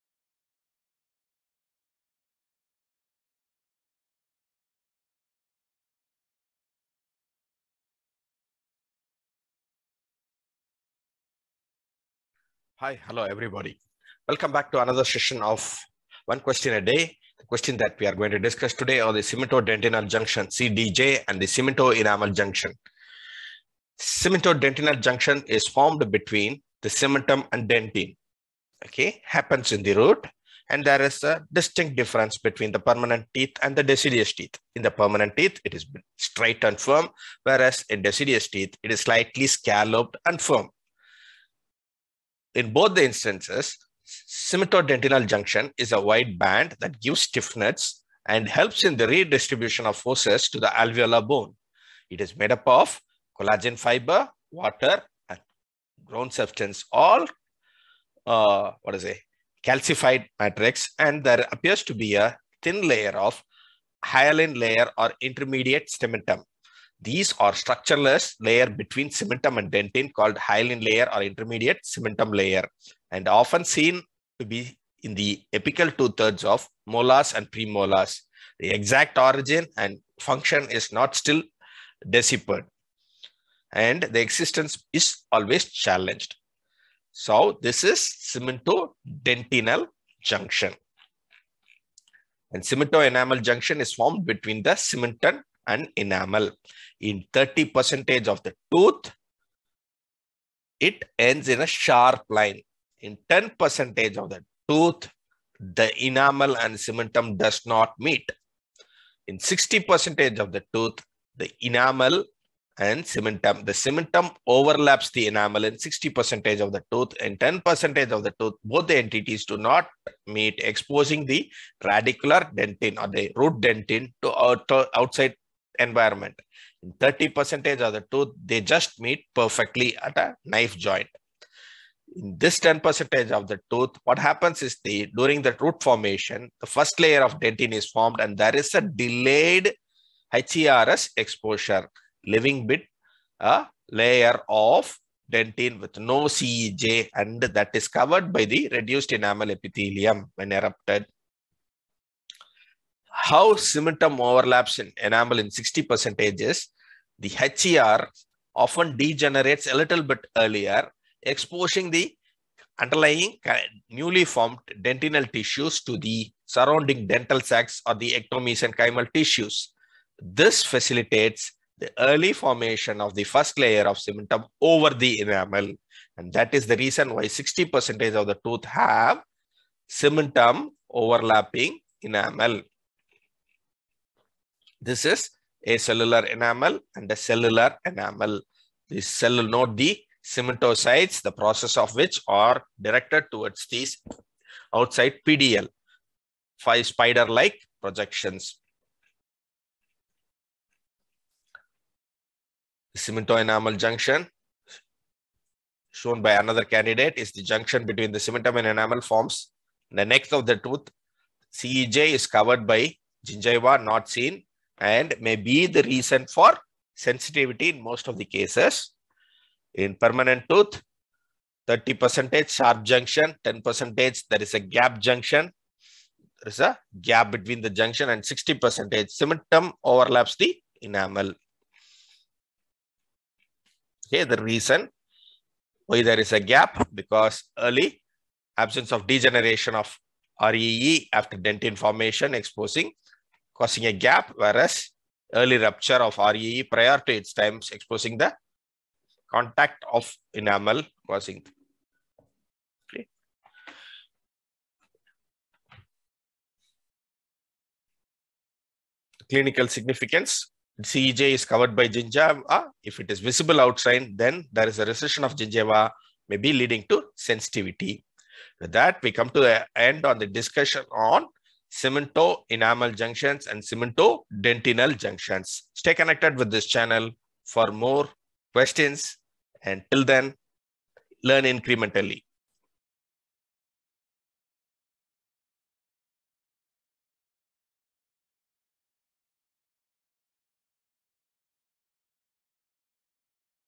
How to answer best for the question on cemento-enamel and cementodental junction?
Featuring: Answer sheet of Ms. Vincy Gandhi